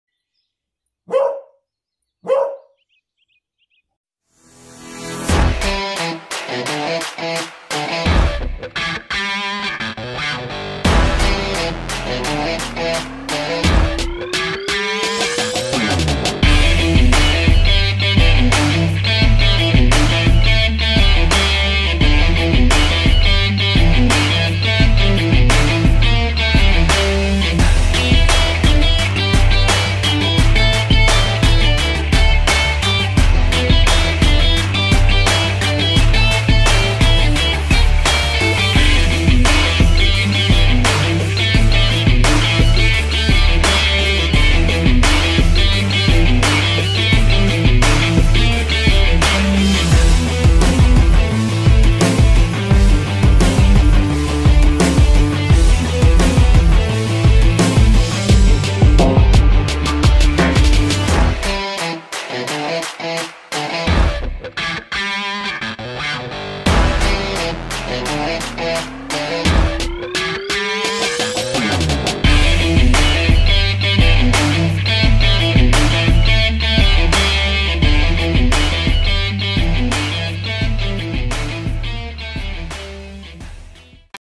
The Slow Lane II Mini Beagle Film II Walking Our Beagles Through By Lanes Near Our Home
Capturing the spirit of the lane and by lane where we live! We love the retro, old world charm and walking through it is just beautiful! We have tried to capture the same for you to see...